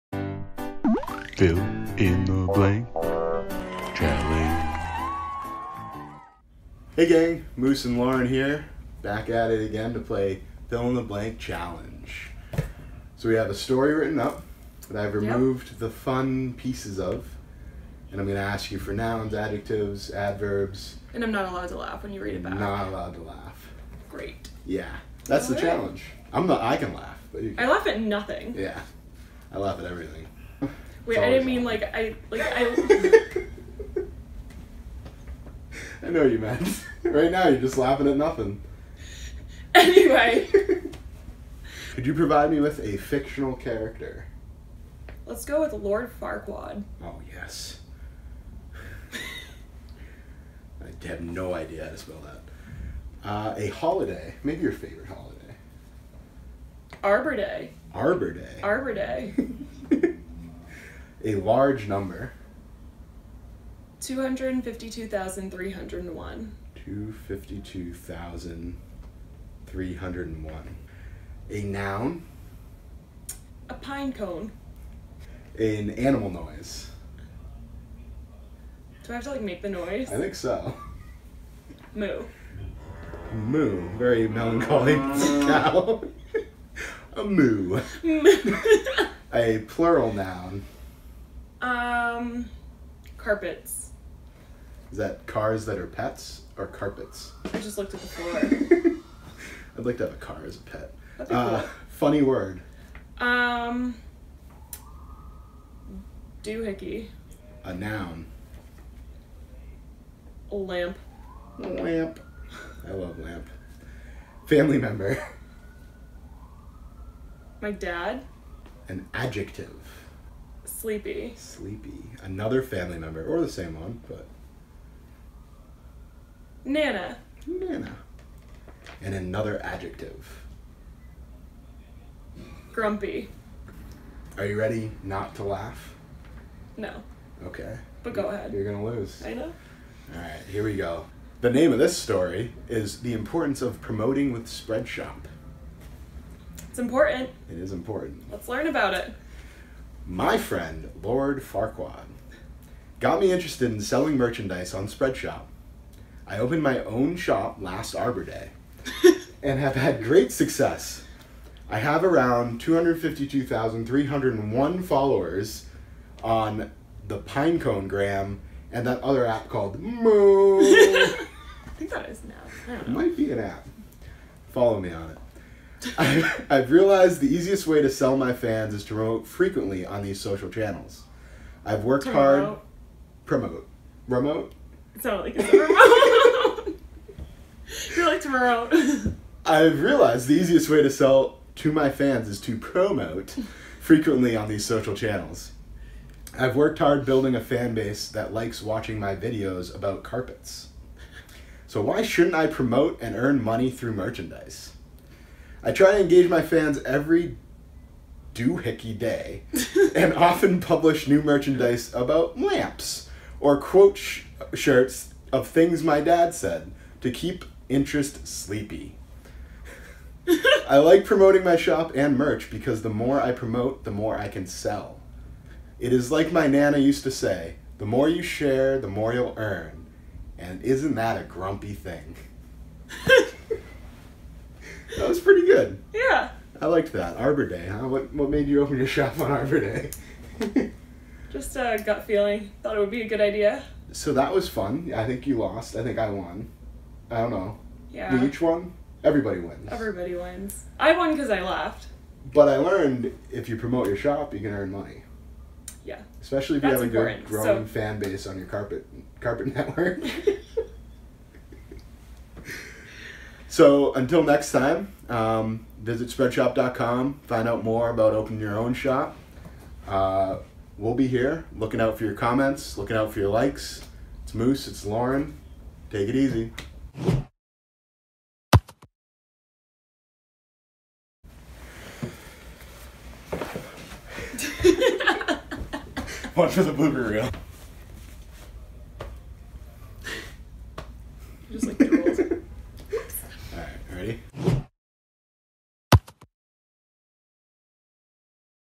Fill in the Blank: Promoting with Spreadshop
BLOOPERS AT THE END!

Join us in a hilarious game of Mad Libs and learn about promoting your Spreadshop!

Open your own free eCommerce store to create and sell t-shirts and other branded merchandise with Spreadshop!